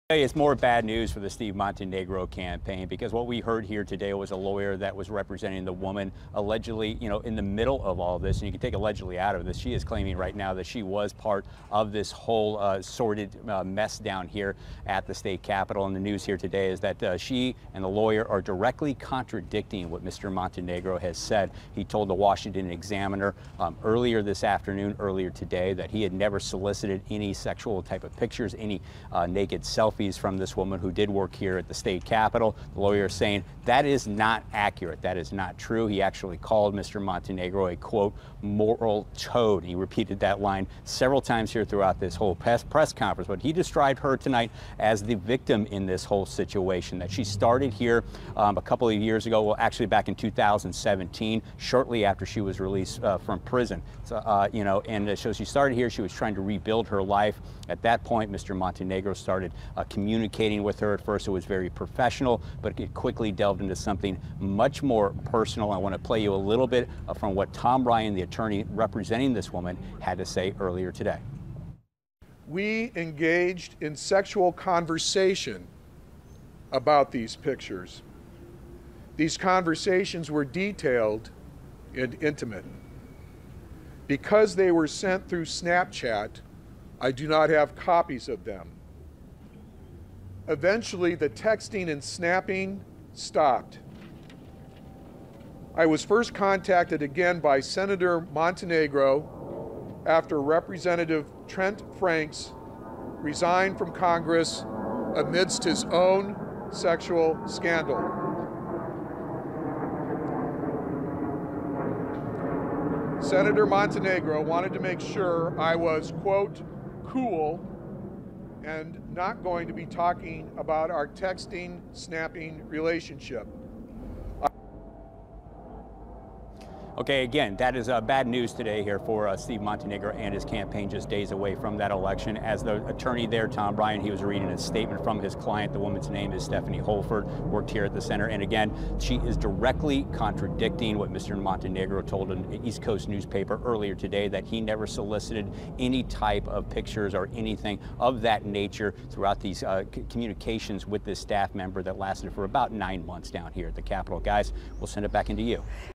VIDEO: Attorney releases Montenegro texts with staffer
The lawyer representing a staffer that sent racy texts to Congressional District 8 candidate Steven Montenegro spoke about the relationship and released the text messages.